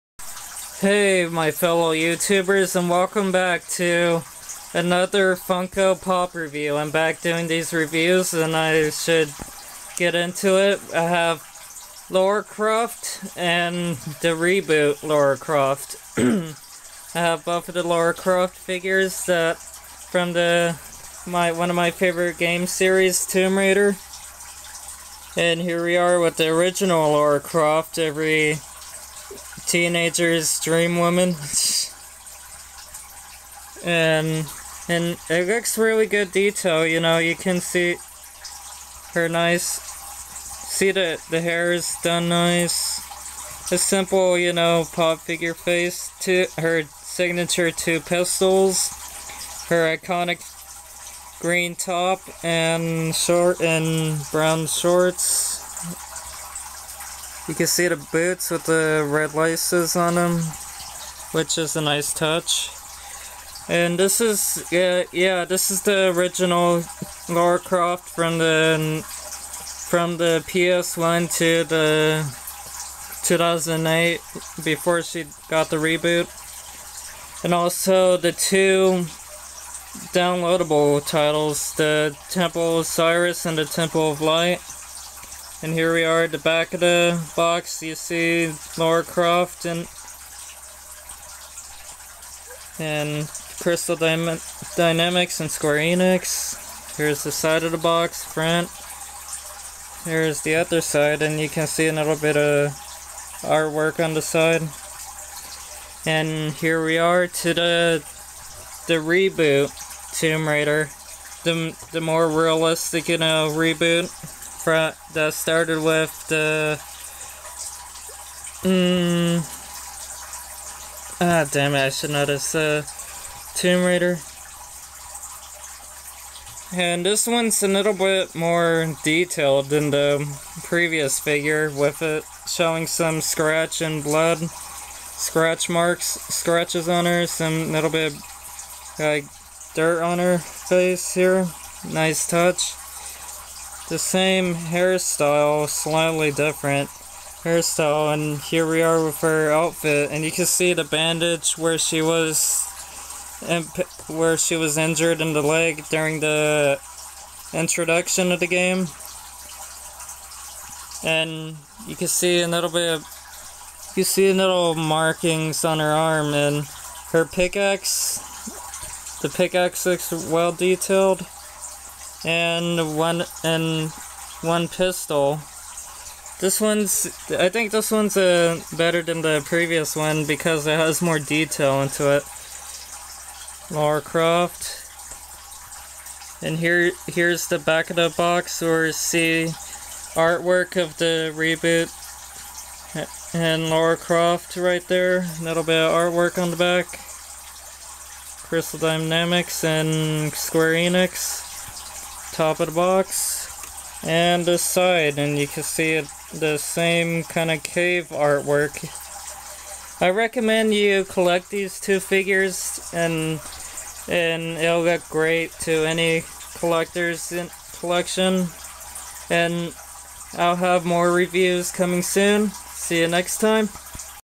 Pop Vinyl Tomb Raider Lara Croft Review Two Pieces of Treasure
Yo its Mario Gamepro and this time I'm doing a review on the Pop Vinyl original and reboot Lara Croft figures these are amazing figures nicely detailed especially the Tomb Raider reboot version with the dirt scratches bandages and pickaxe but the original is just as great with the classic green top brown shorts and her dual pistols I highly recommend these to any Playstation and Tomb Raider fan so enjoy ideas suggestions and requests anytime :)